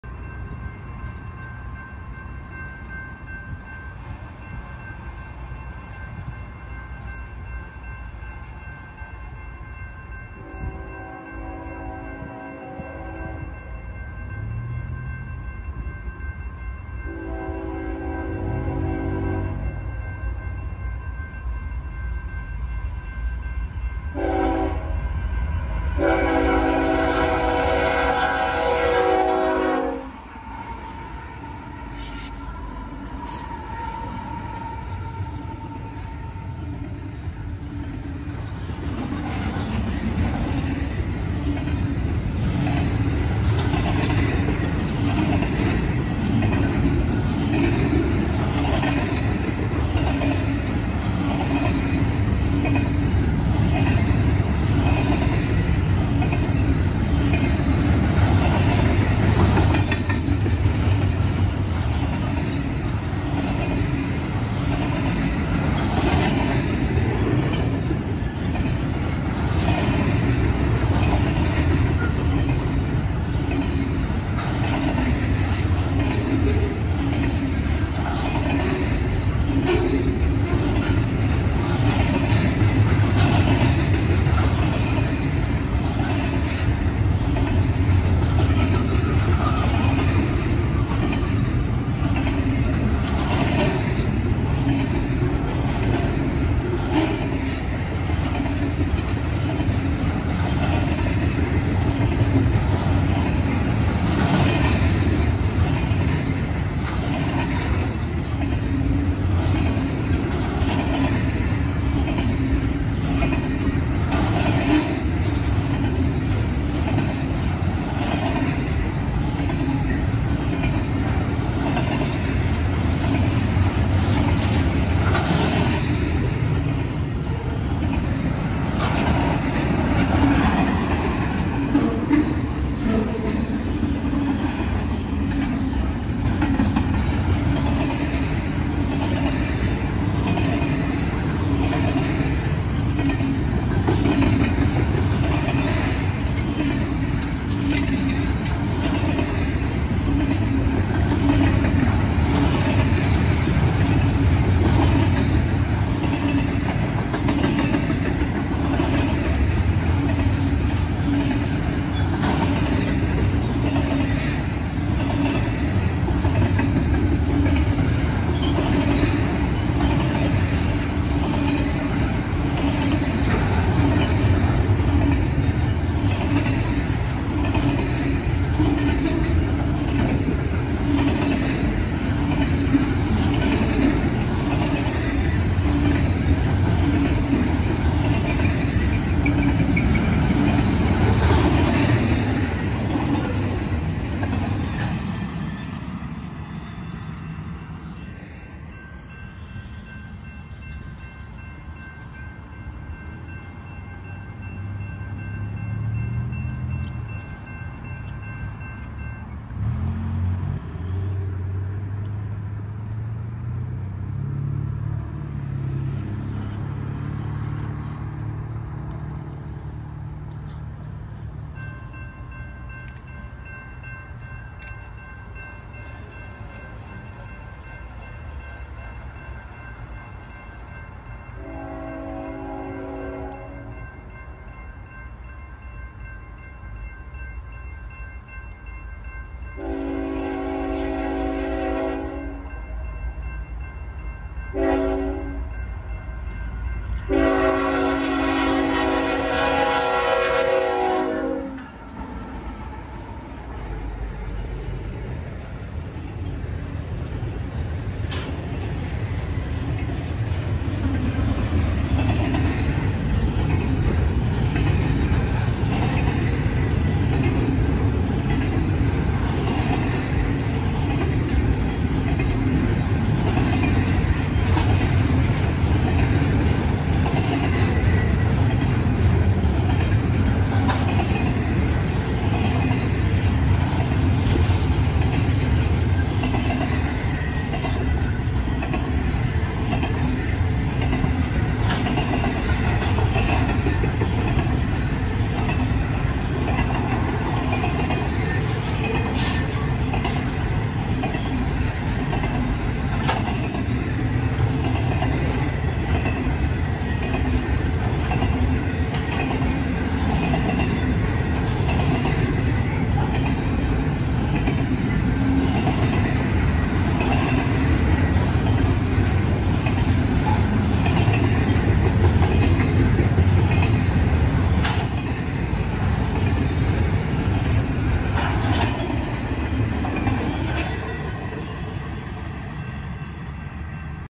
Union Pacifc Trains in Redlands, CA
Here are a couple of fast Z trains on the Sunset Route in San Timoteo Road in Redlands, CA